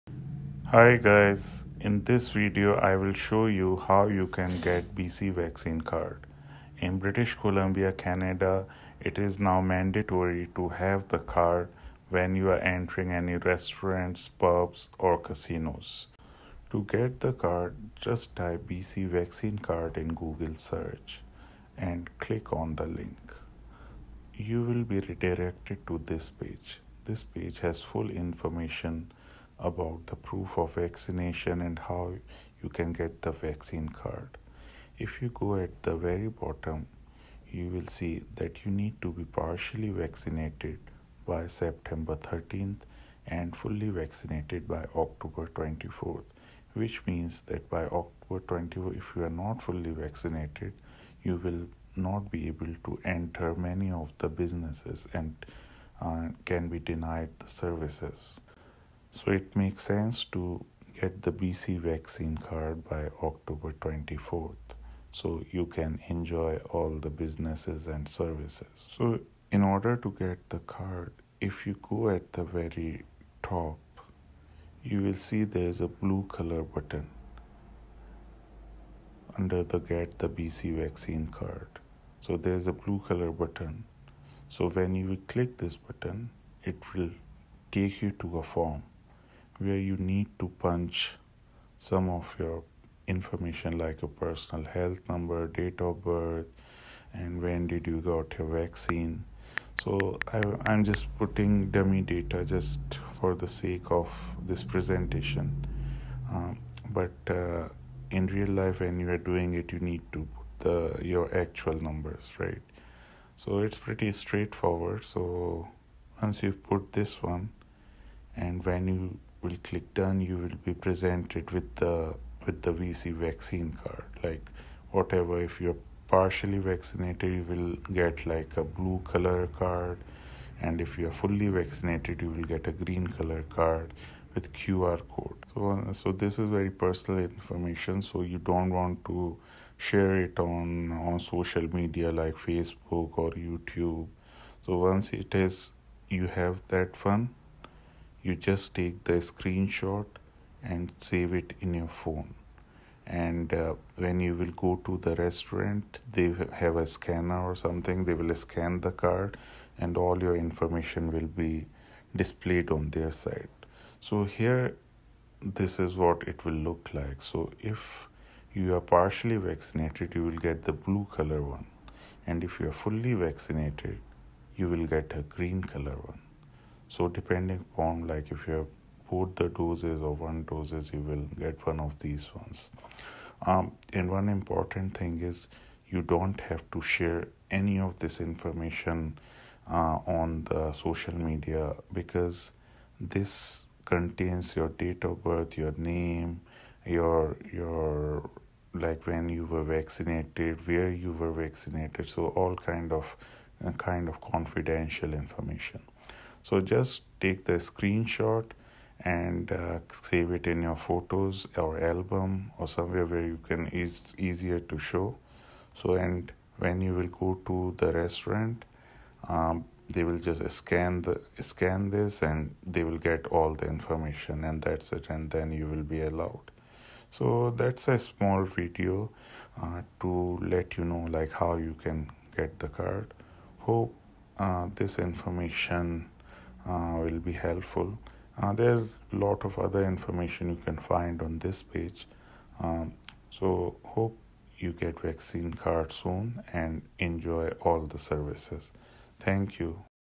BC Vaccine Card
This video shows how to get BC vaccine card